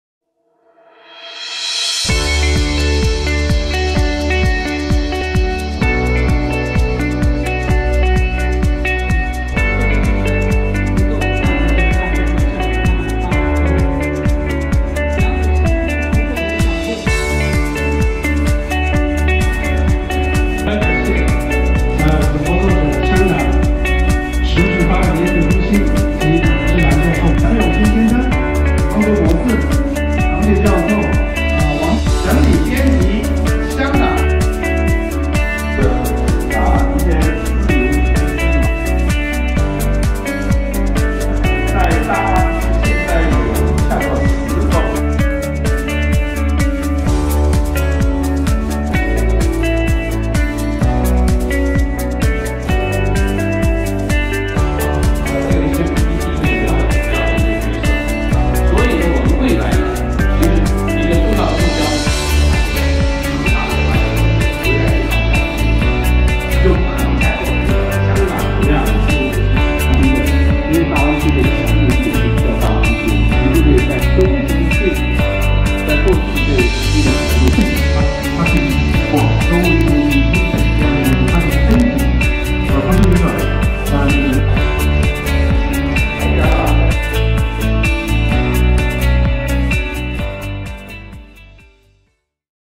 「香港未來與粵港澳大灣區」新書發佈會暨公開講座
本社出版《粵港澳大灣區：合作策略與香港未來》、《粵港澳大灣區城市群概覽2017》兩本重要出版物，於2018年3月22日舉辦新書發佈會暨公開論壇，邀請公眾人士與新書的作者、編者，以及城大師生代表一同參加。